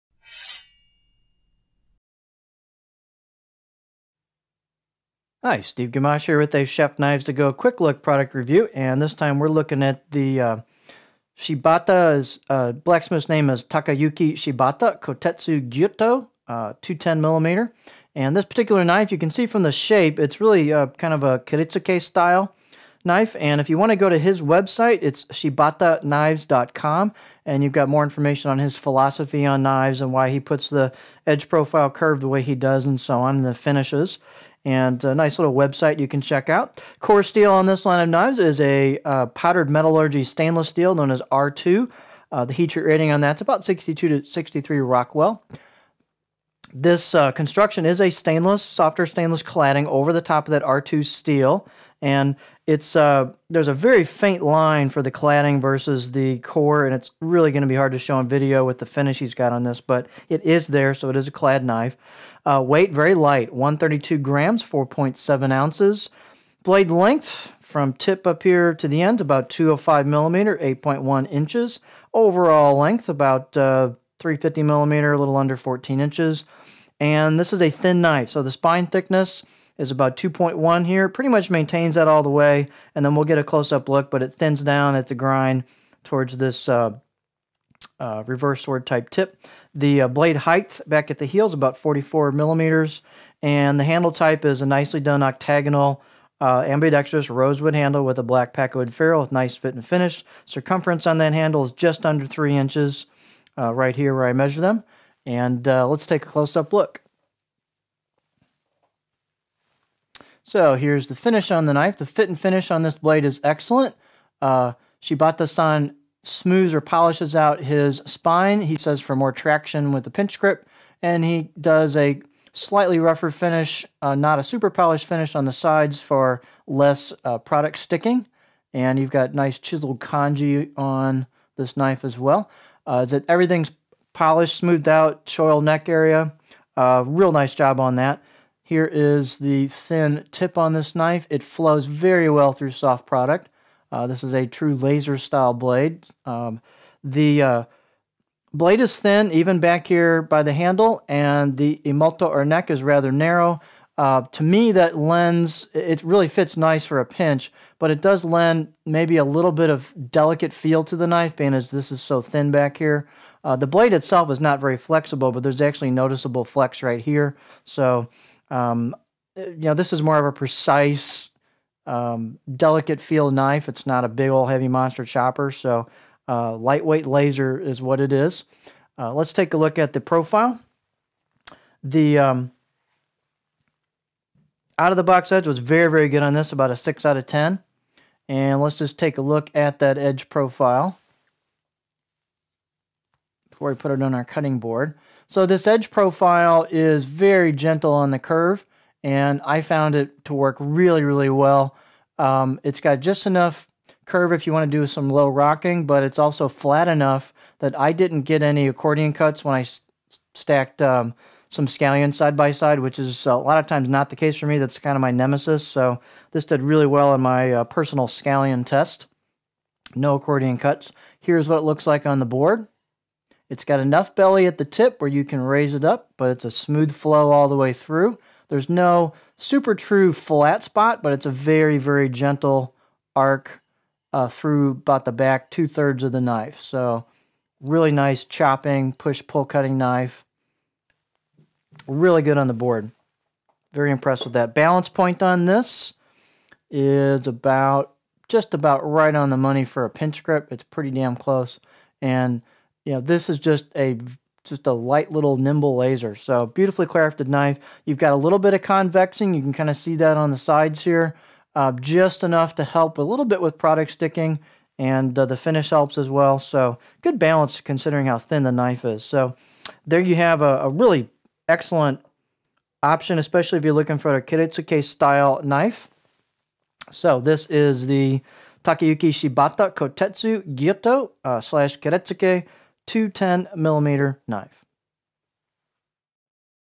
Shibata Kotetsu Gyuto 210mm Quick Look
Here is a quick product demo of the Shibata Kotetsu Gyuto 210mm.